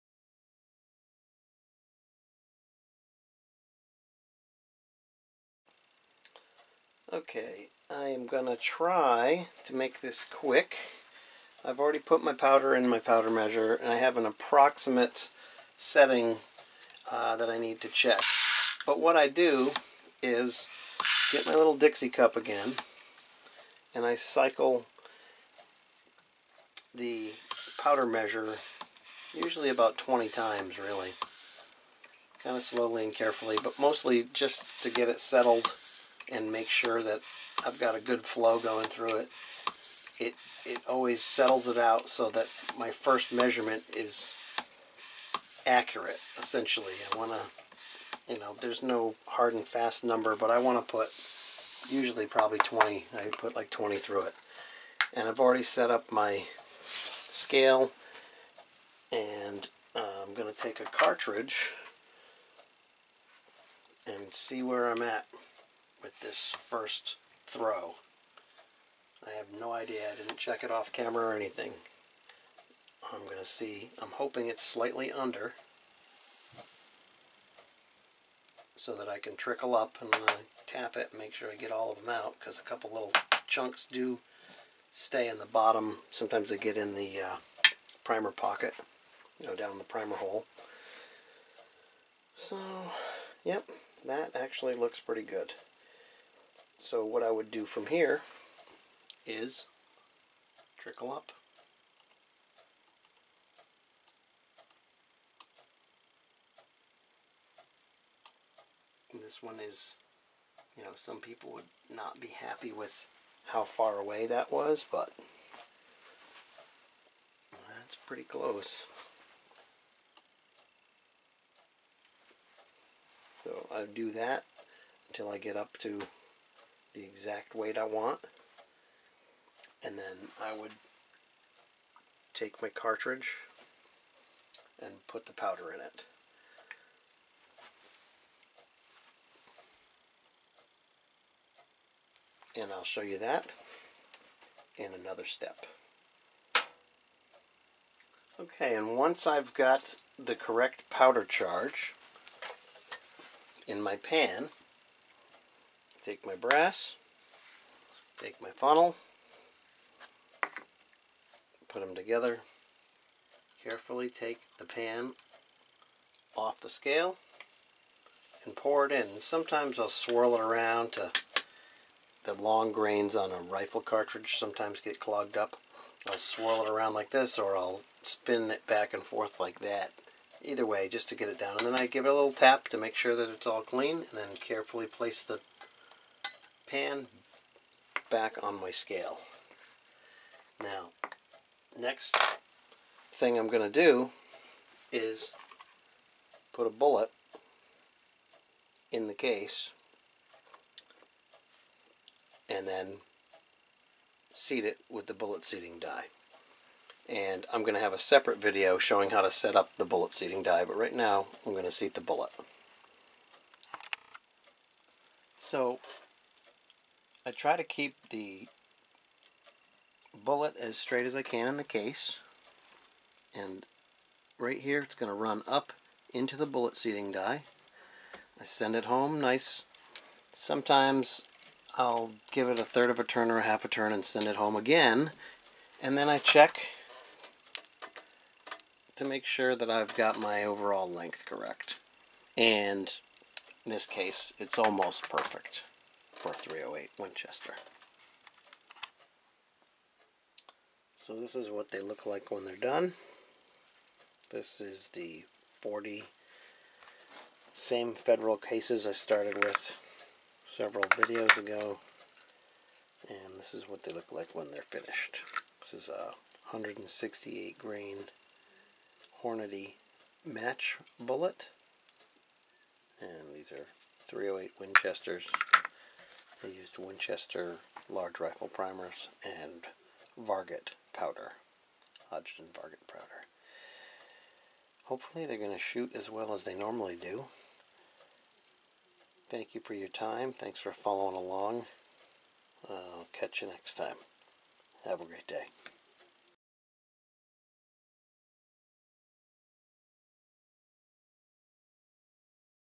Charging the Case and Seating the Bullet
Charging the case and seating the bullet in a .308 Winchester cartridge. Using a Lee perfect powder measure and bullet seating die. Finishing the .308 Winchester cartridges started in the Lyman prep tool video.